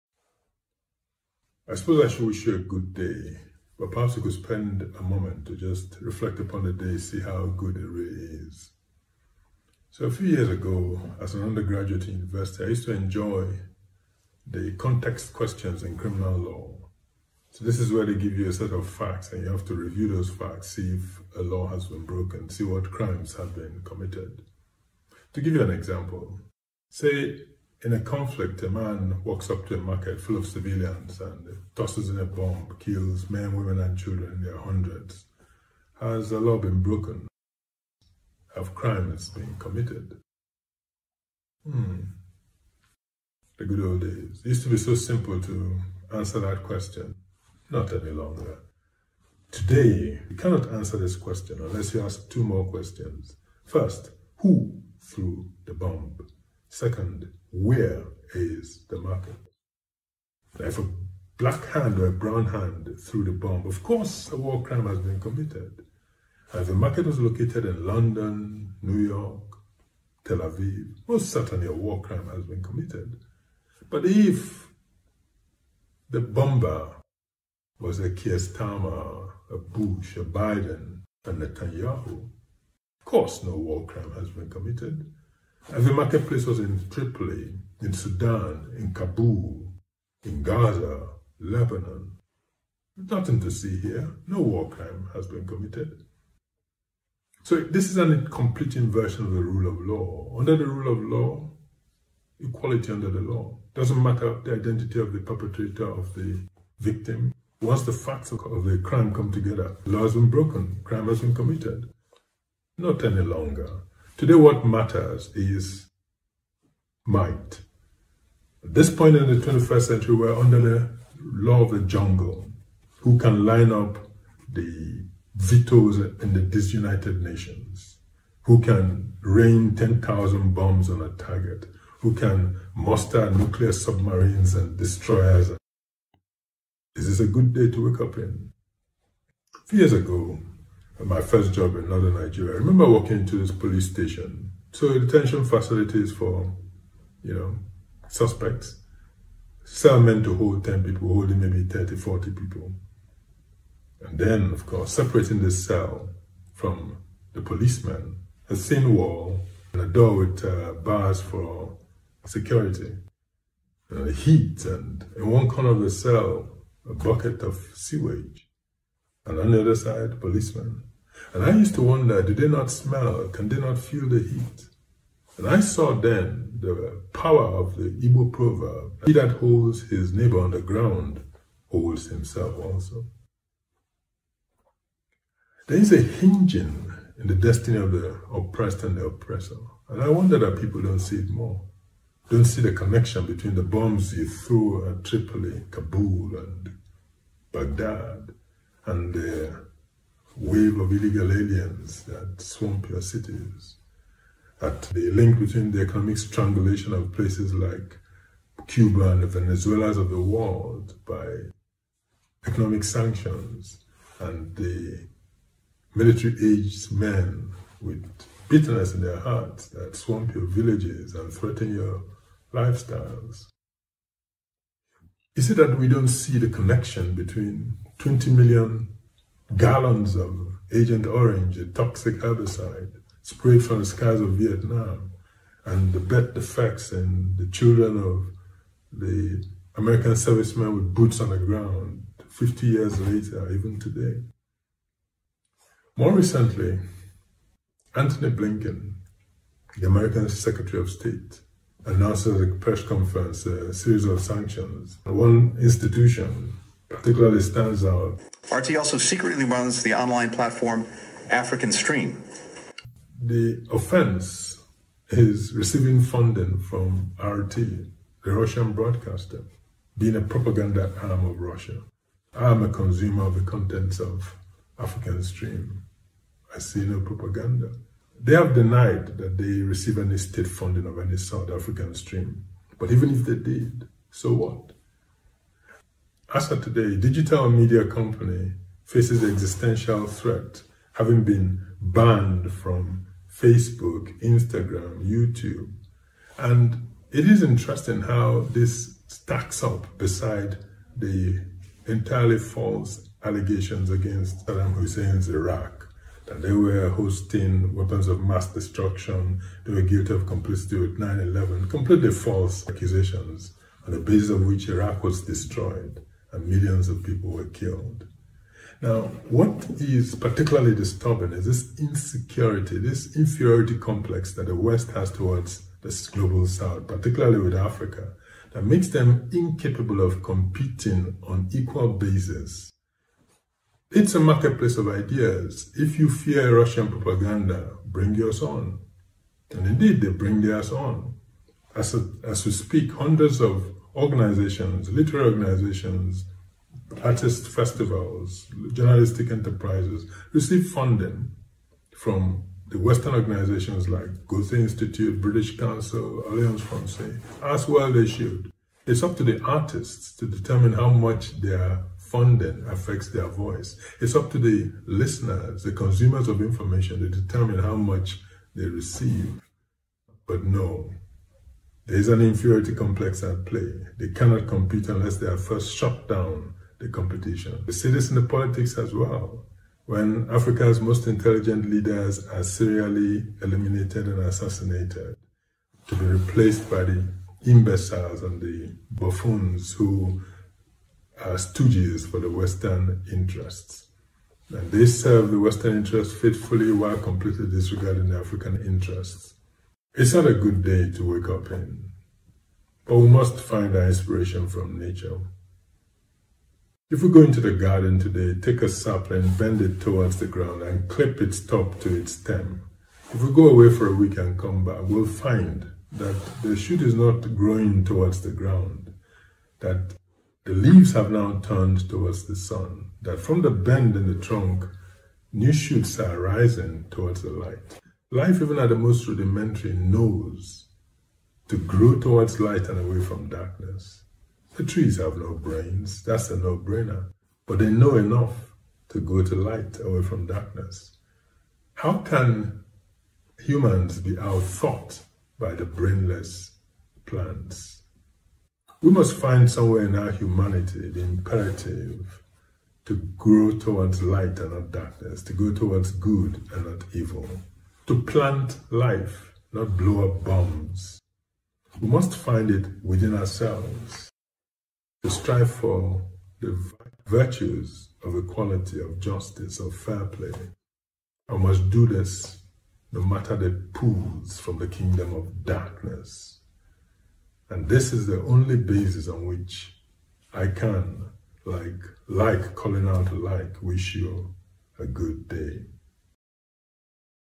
Seize the Day
In which I consider the hypocrisies embedded in the "Pax Babaria", which is currently our world system.. Time to... carpe diem.